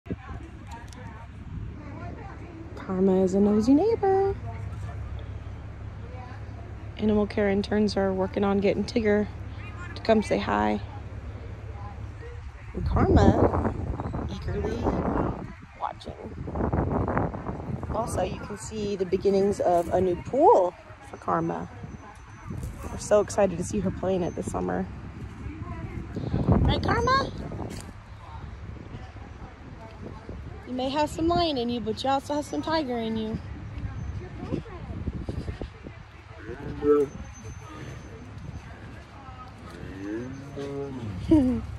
Karma Liger is a Nosey Neighbor
HAPPY TUESDAY!! Karma Liger is being a nosey neighbor as she watches animal care staff and interns working with Tigger Tiger.  You can also see the work has begun on Karma's new pool.